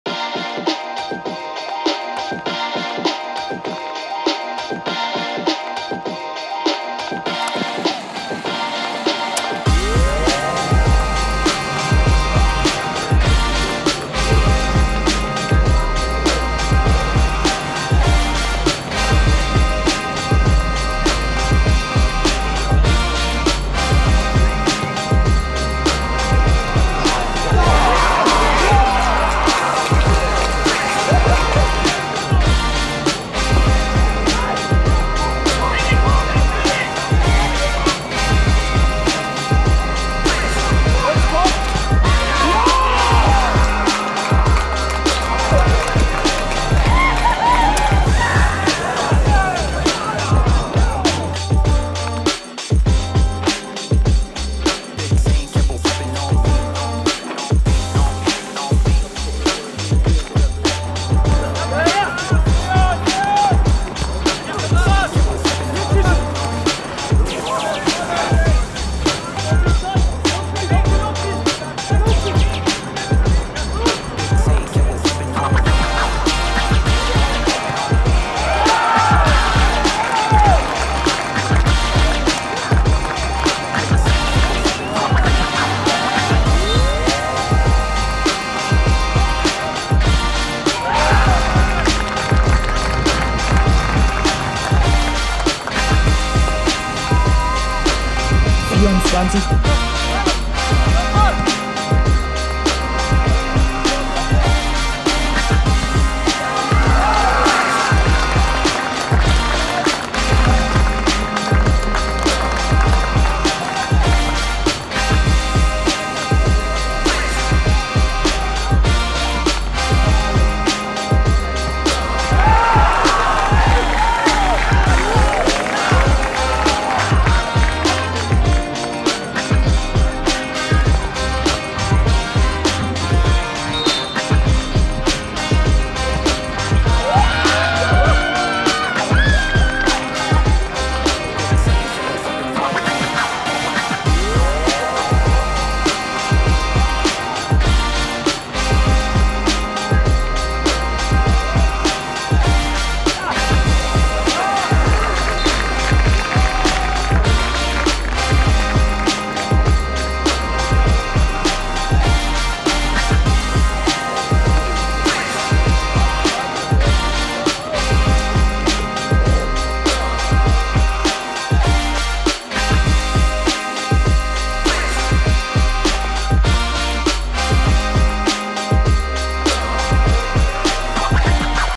Die NLZ-Torparade: 12. - 13. Februar
Die Tore der Nachwuchsteams vom vergangenen Wochenende.